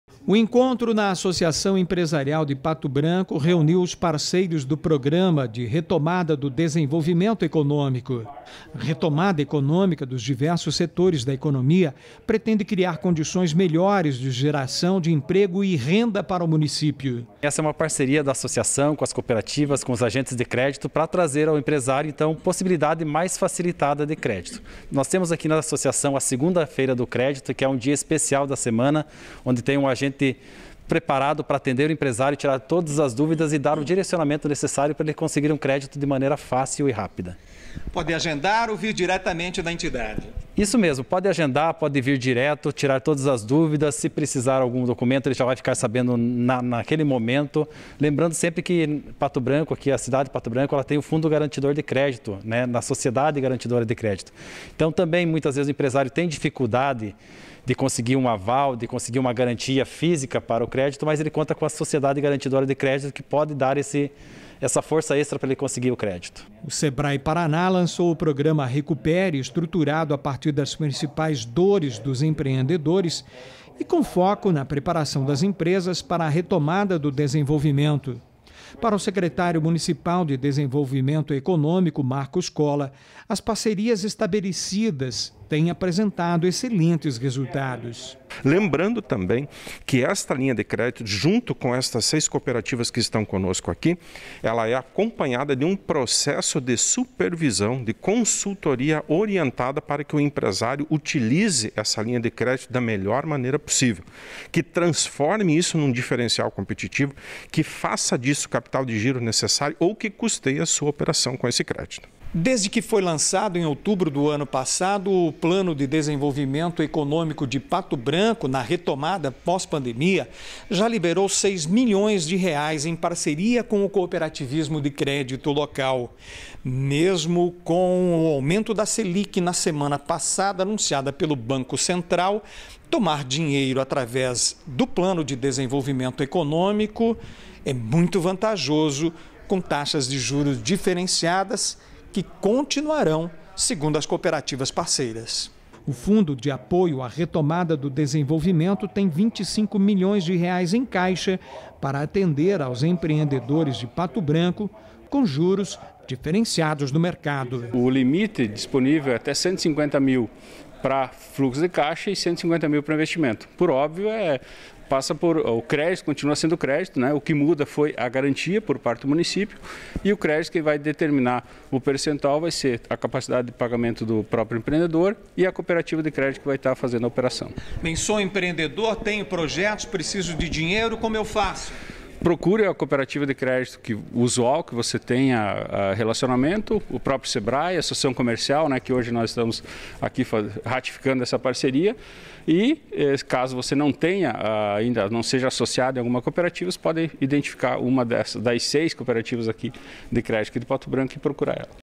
Plano de recuperação das empresas que sofreram na pandemia
Economia: A linha de crédito já liberou em Pato Branco mais de seis milhões de reais.